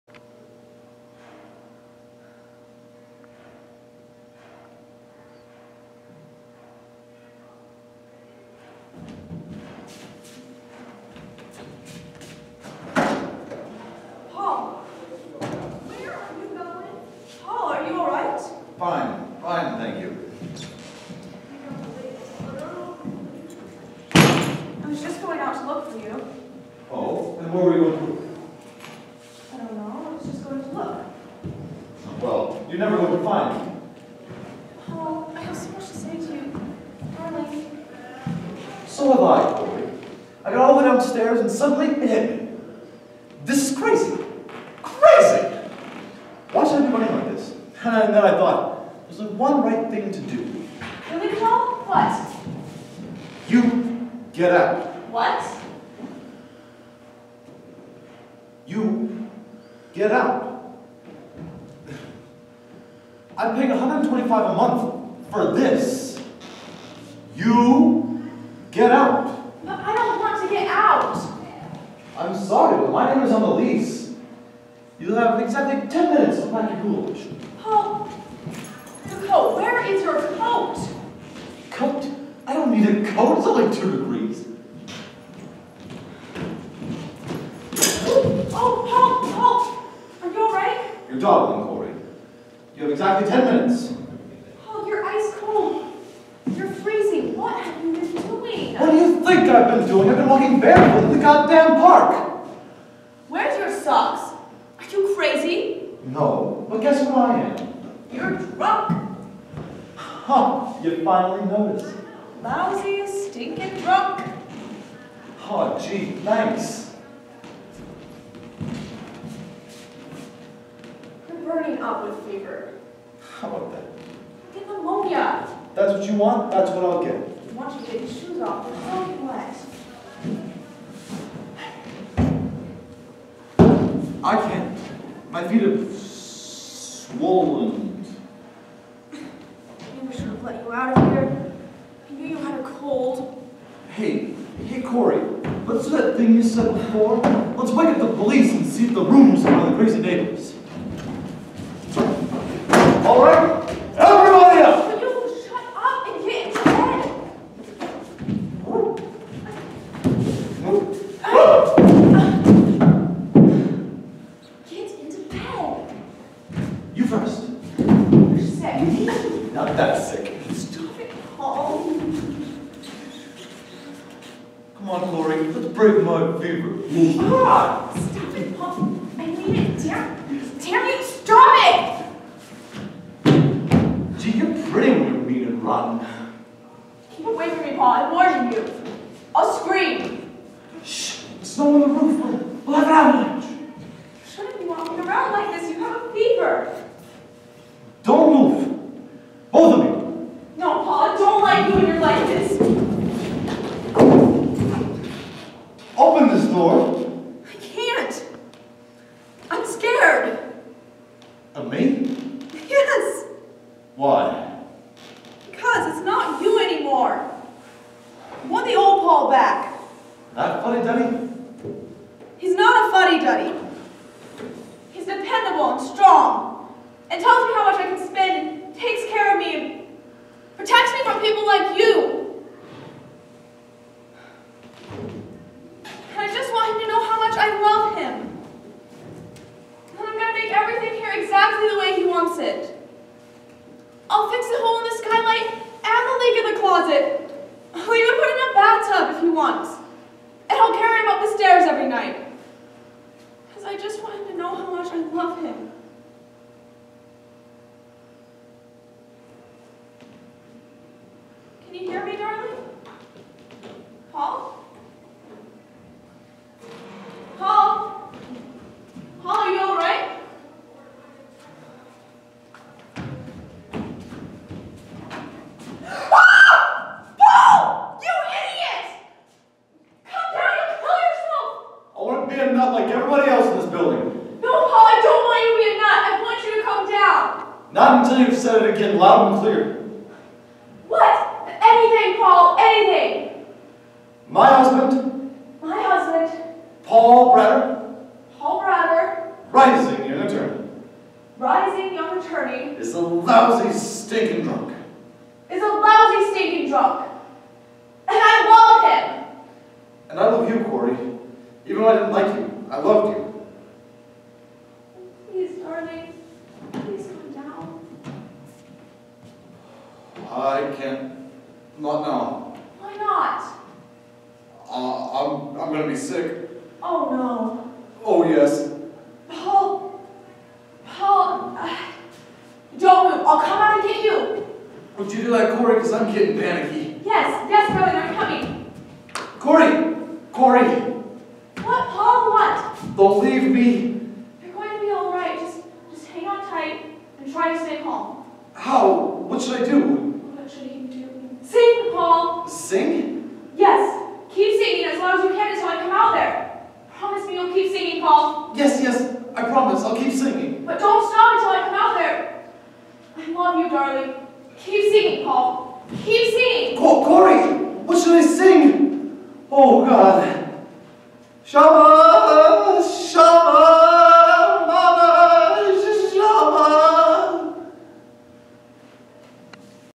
Barefoot in the Park
my scene partner nick and I's Barefoot in the Park. please take it easy on us we are learning! :)